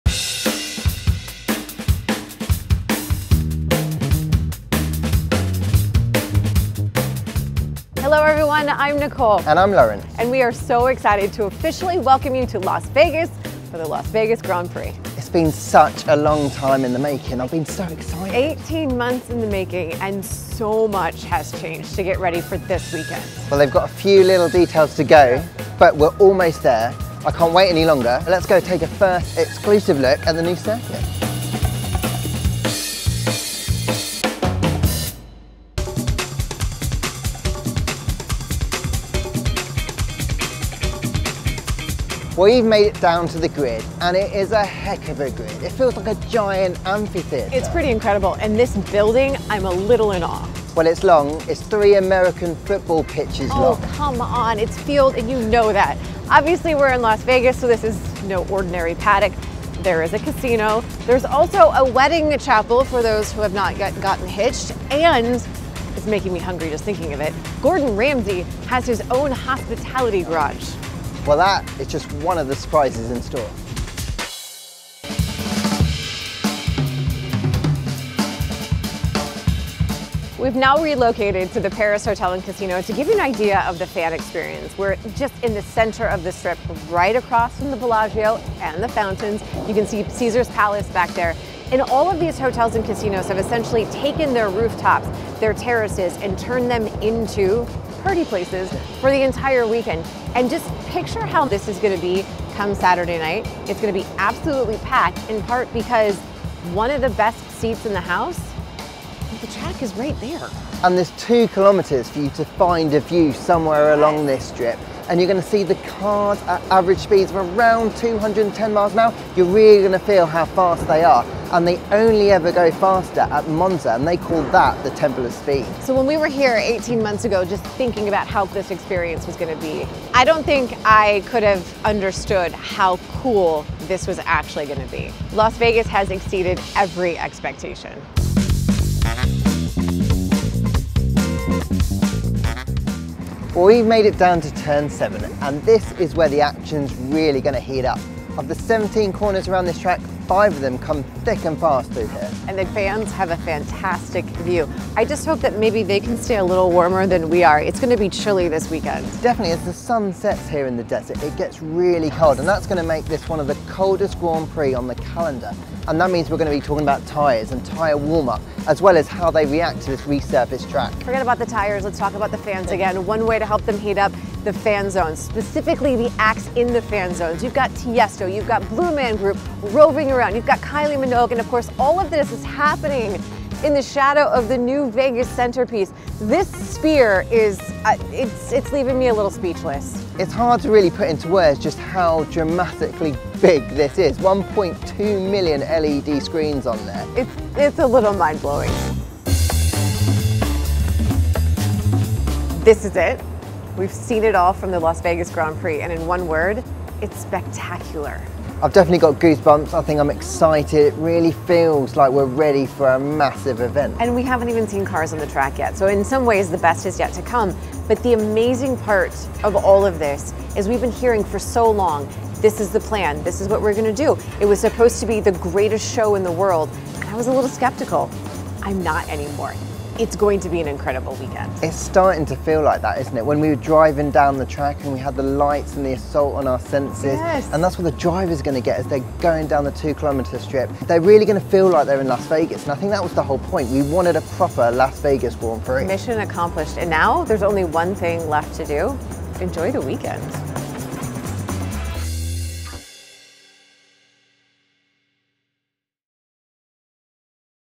A First Look At The Las Vegas Strip Circuit! | 2023 Las Vegas Grand Prix
Welcome to the Las Vegas Strip Circuit! Lawrence Barretto and Nicole Briscoe look at every twist and turn of the exciting new street circuit which runs down the Las Vegas Strip.